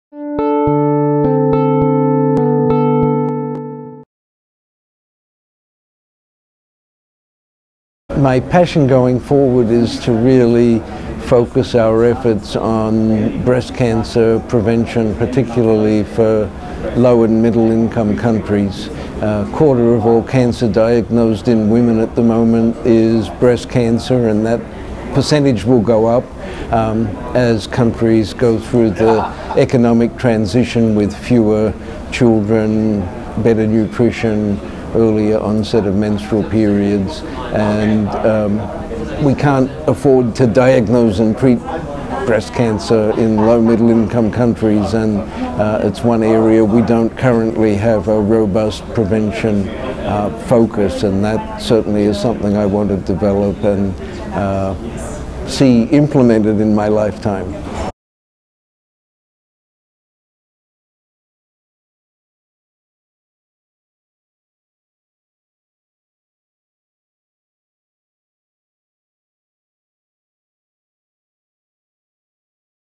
Dr. Colditz on Breast Cancer Prevention in Low and Middle Income Countries
Graham Colditz, MD, DrPH, associate director, Prevention and Control, Alvin J. Siteman Cancer Center, discusses the need to focus on breast cancer prevention in low and middle income countries.